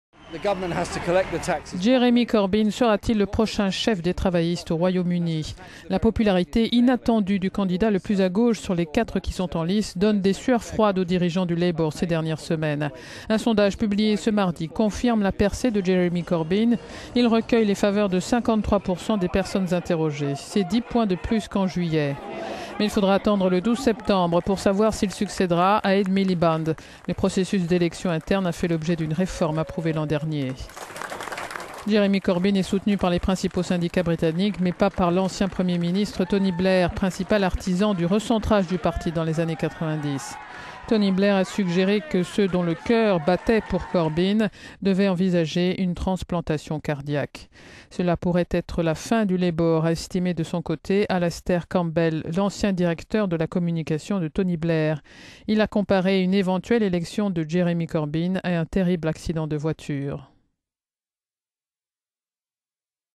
Succession au Labour britannique : popularité grandissante du candidat de l'aile gauche
Jeremy Corbyn sera-t-il le prochain chef des travaillistes au Royaume-Uni ? La popularité inattendue du candidat le plus à gauche sur les quatre qui sont en lice donne des sueurs froides aux dirigeants du Labour ces dernières semaines. Un sondage publié ce mardi confirme la percée de Jeremy Corbyn. Il recueille les faveurs de 53 % des personnes interrogées. C'est dix points de plus qu'en juillet. Mais il faudra attendre le 12 septembre pour savoir s'il succèdera à Ed Miliband. Le processus d'éle…